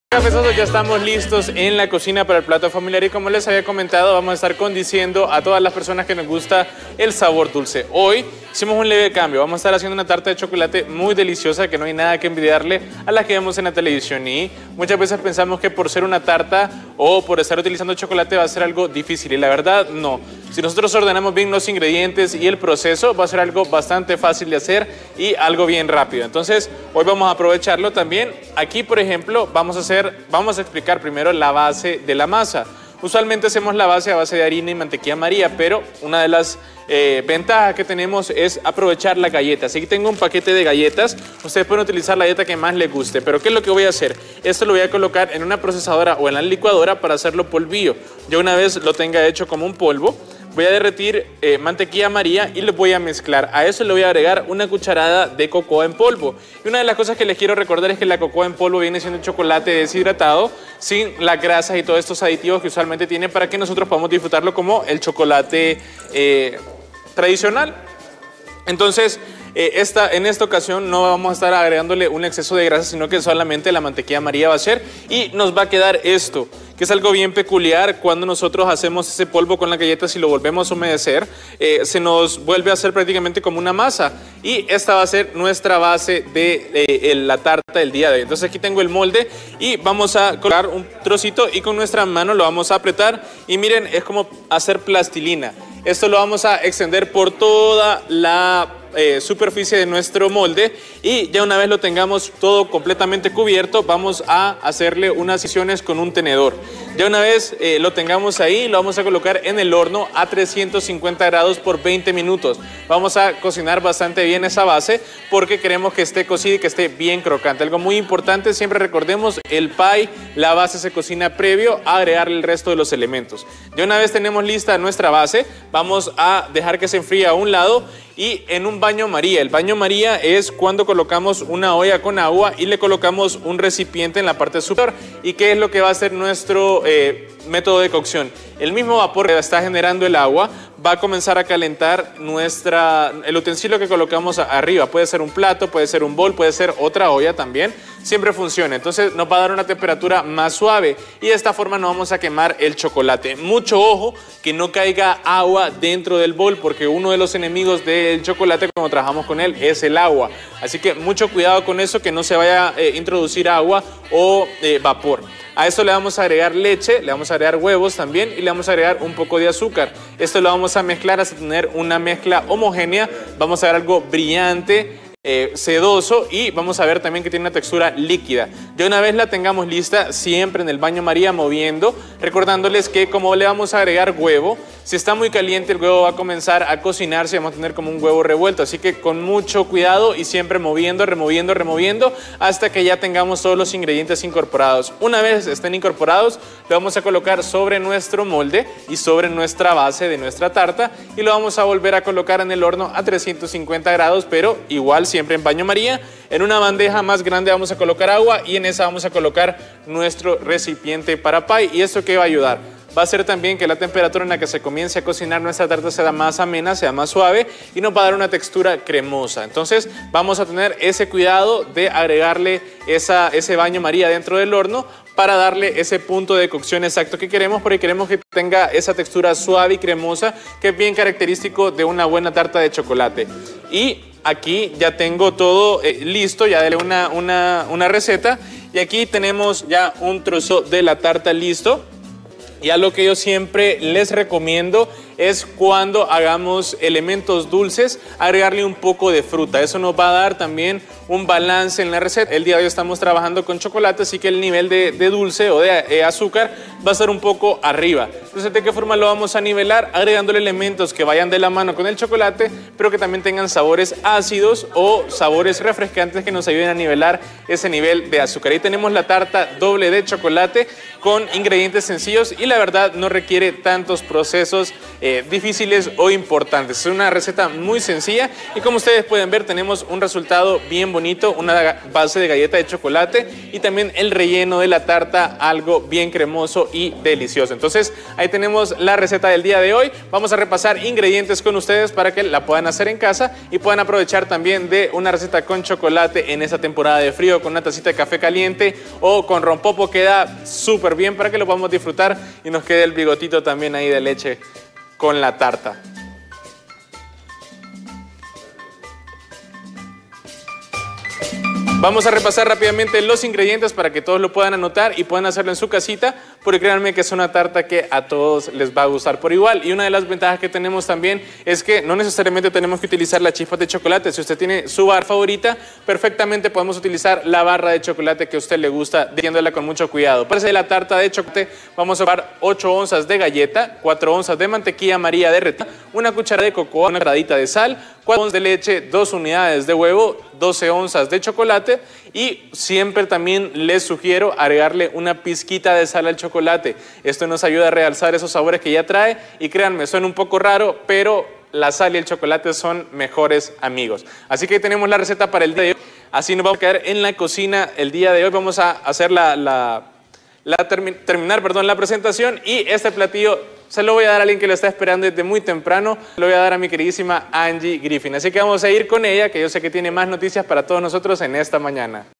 Aprende a preparar una tarta de chocolate
El chef no enseñará a prepara una rica tarta de chocolate, un postre delicioso y perfecto para estos días. No te pierdas de esta gran receta.